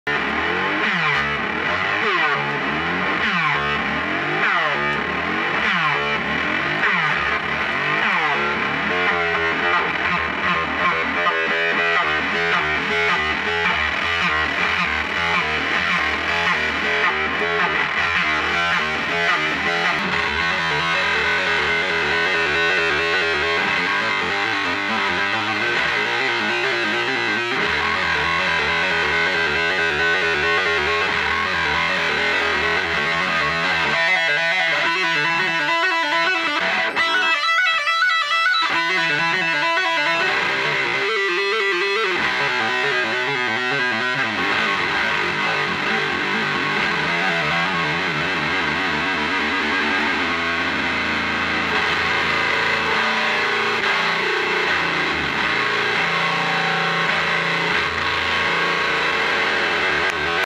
Visit from Homenoise effects, testing the Giant Hogweed Fuzz
Here you can hear the Homenoise Black Lodge delay into the drolo Giant Hogweed Fuzz